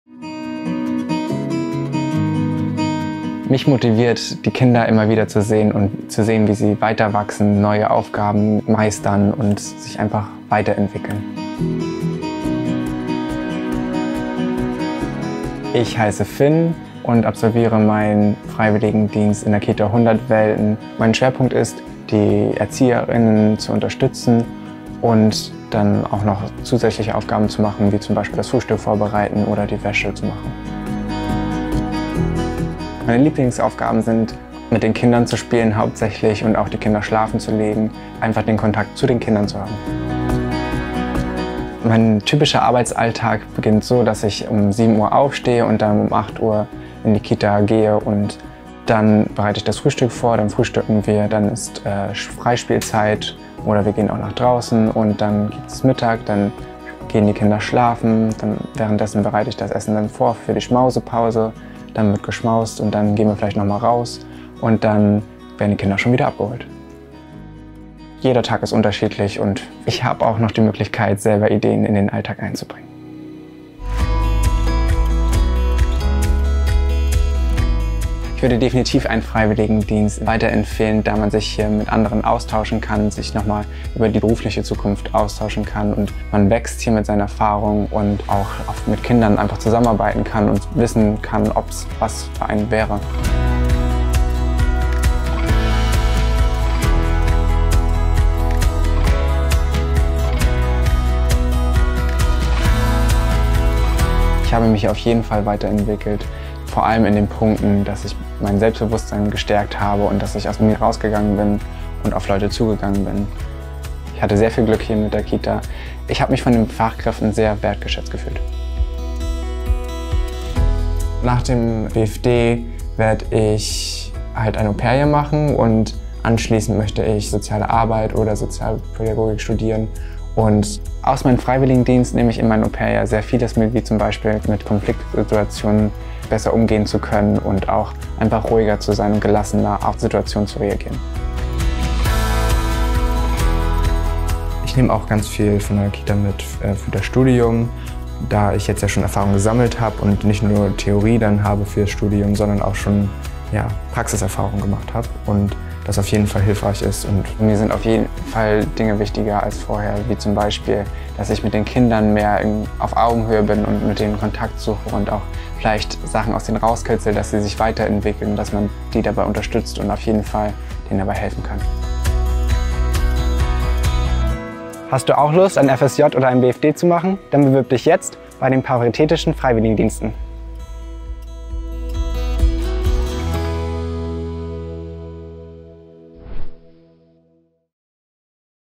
Freiwilligendienst (FSJ/BFD) in einer Kindertagesstätte
Finn macht seinen Bundesfreiwilligendienst (BFD) in einer Kita. Im Interview erzählt er, was seine täglichen Aufgaben sind und inwieweit der Freiwilligendienst seine persönliche und berufliche Zukunft beeinflusst hat.

Du würdest auch gerne einen Freiwilligendienst im Saarland oder Rheinland-Pfalz machen?